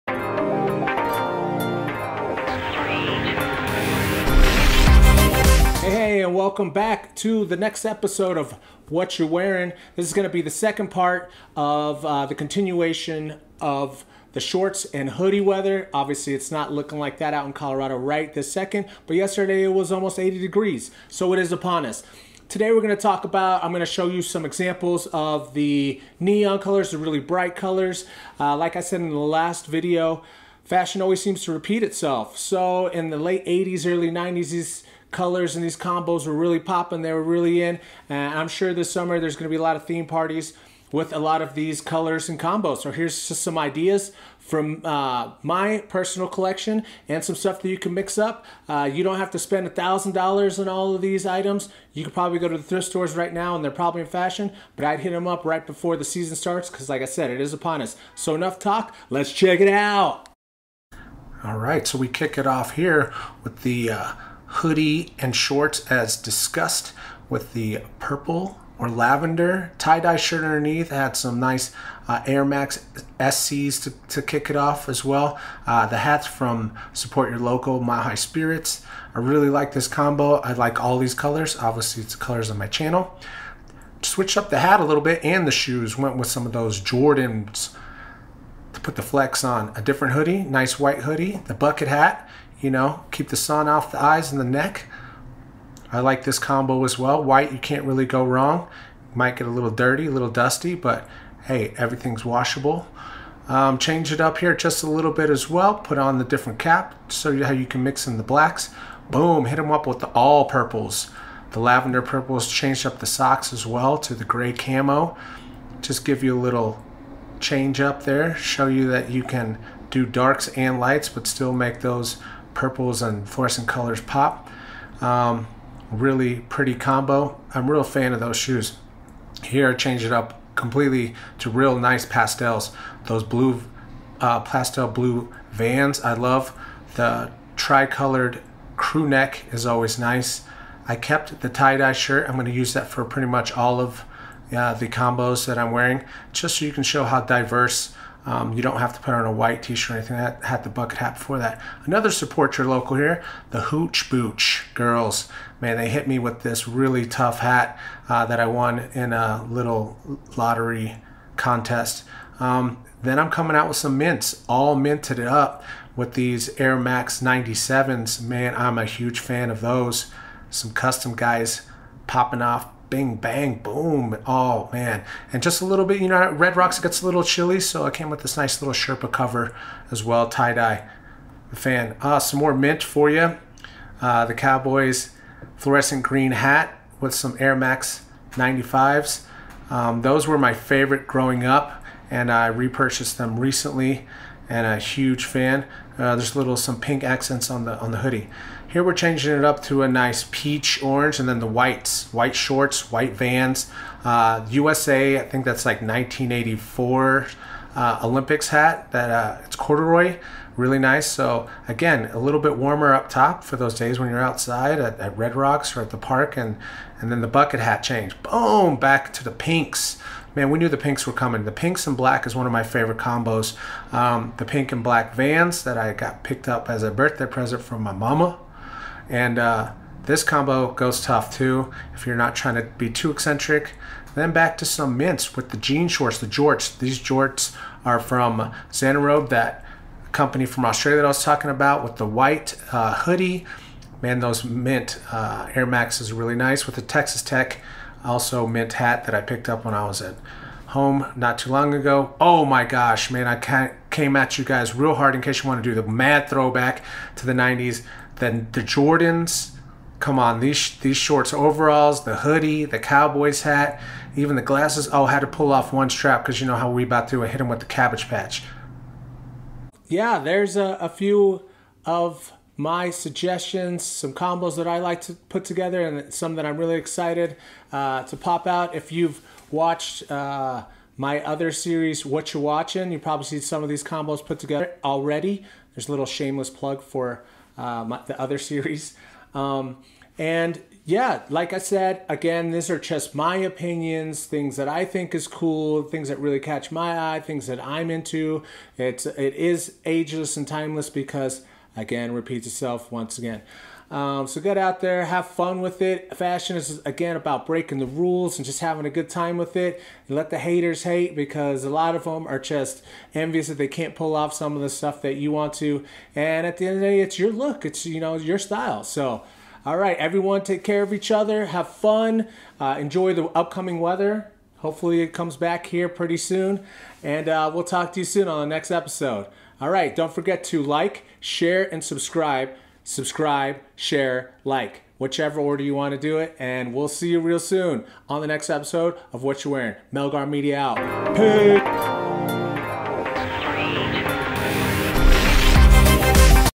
Whatcha Wearin Ep 2
Ayeeee, Part Deux of the Shorts & Hoodies combos! Enjoy the brighter colors this go round!

All hoodies, crewnecks, and shorts are from H&M.
Hats vary, but one is from Mile High Spirts and another is from Hooch Booch. Support your local.